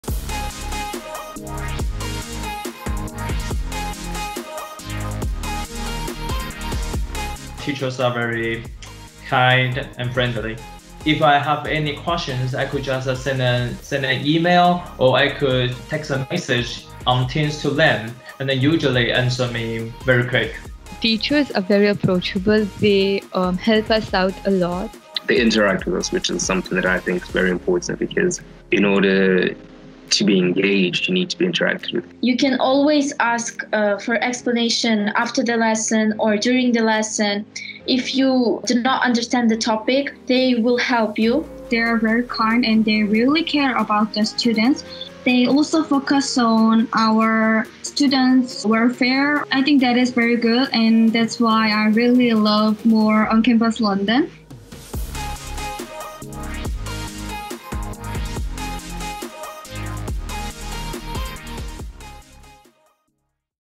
How have ONCAMPUS teachers helped through online learning?
ONCAMPUS students tell us how their teachers have helped them through online learning.